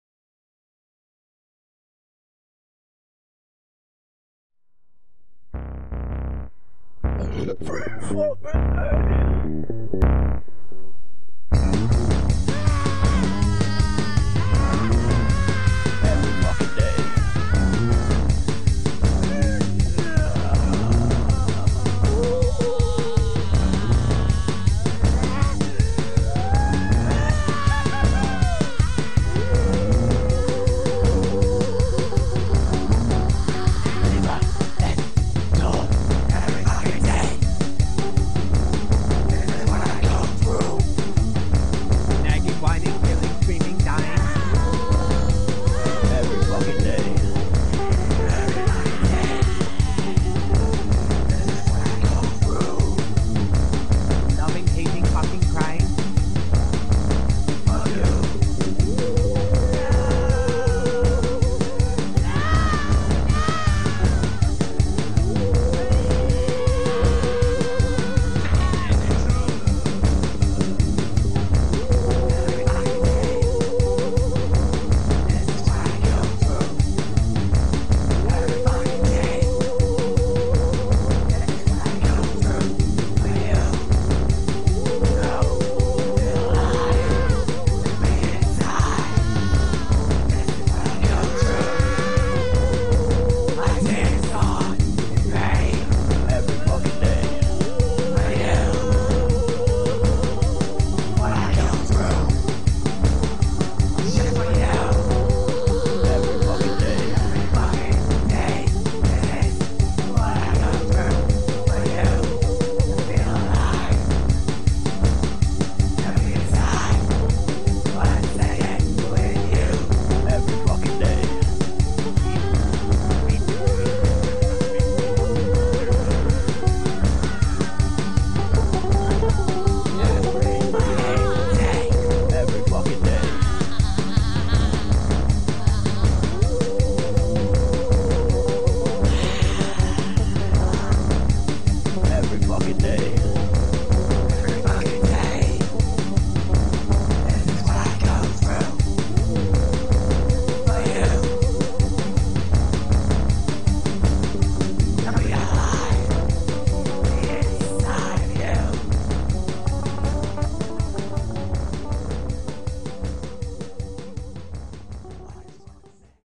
Bad, no budget animation
10 years ago I got a camera and my friend Anson came over. We started messing around and made this. nothing special. music by SILT